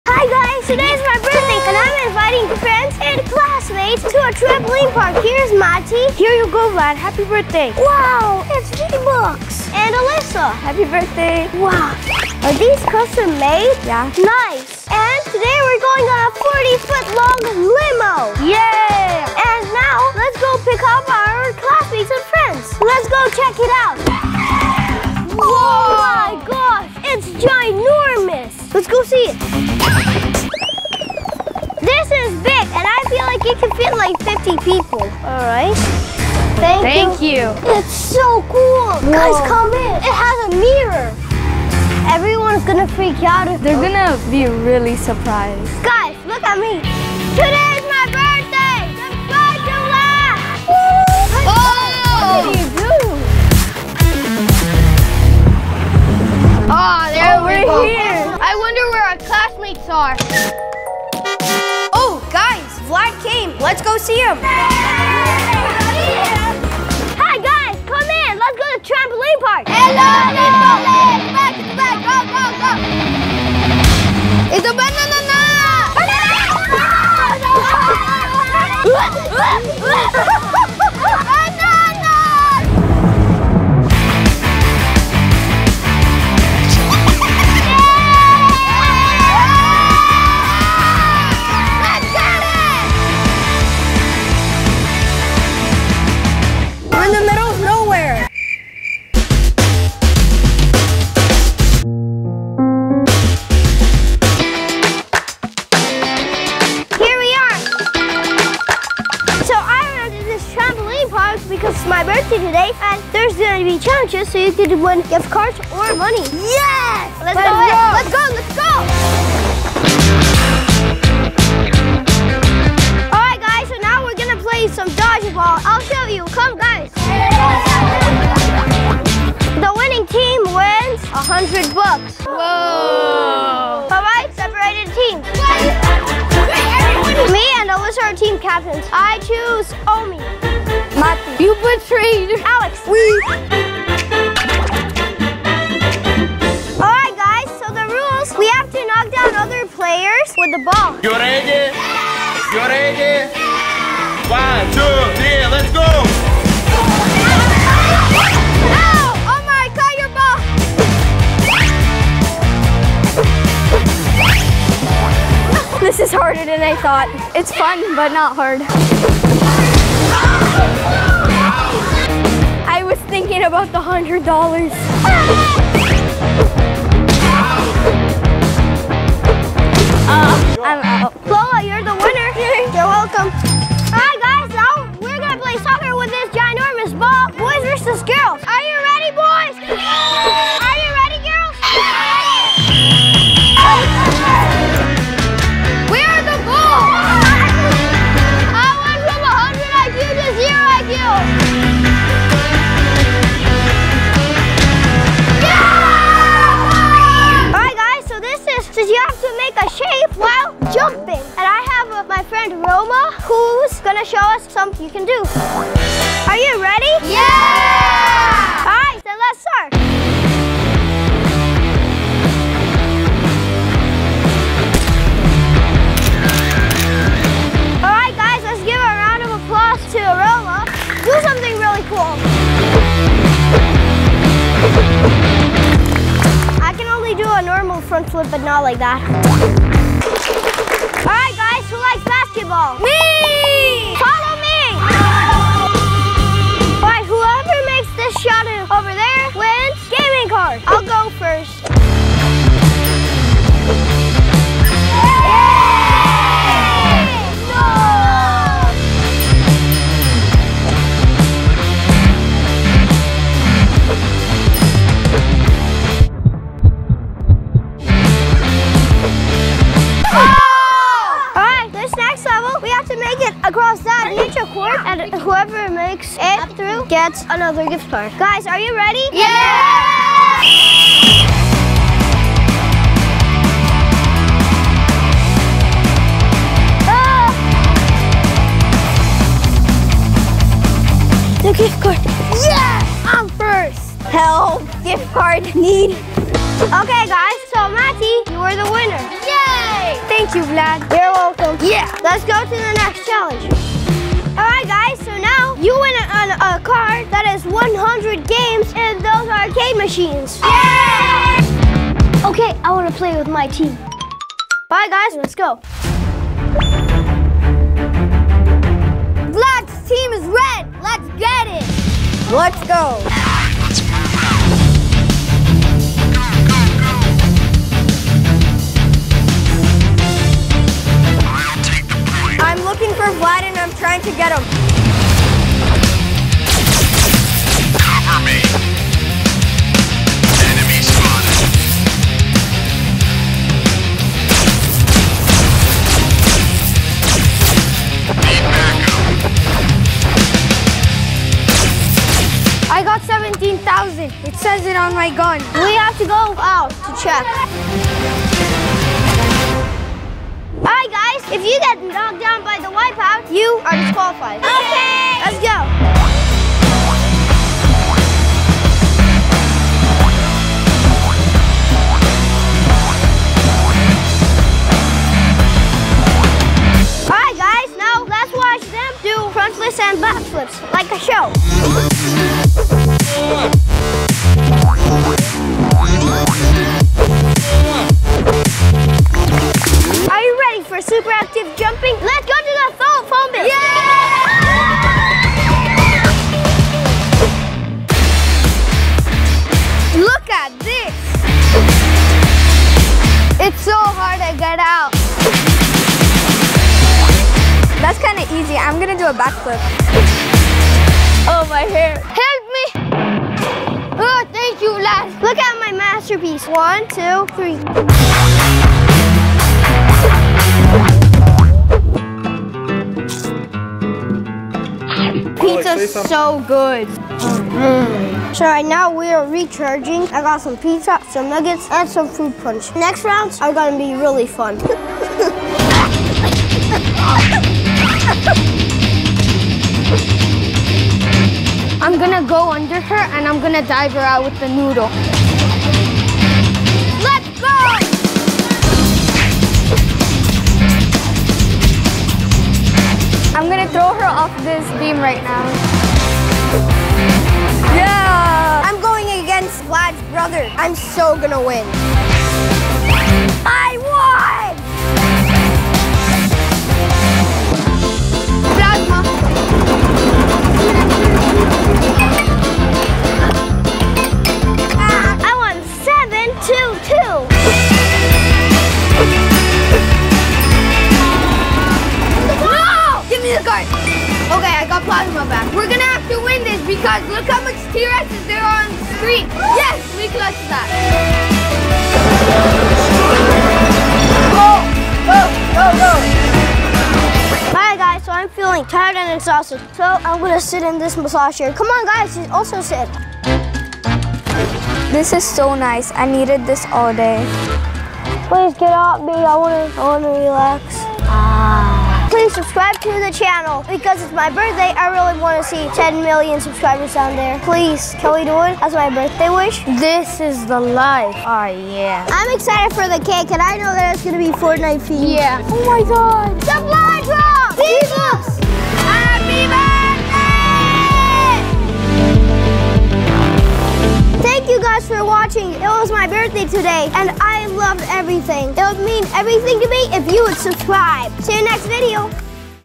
Vlad's Birthday in Extreme TRAMPOLINE PARK | 24 HOUR CHALLENGE
Today is Vlad's Birthday! And he celebrates this day with his friends in Trampoline park!!!
They have to complete the challenges and winner can get gift cards or money.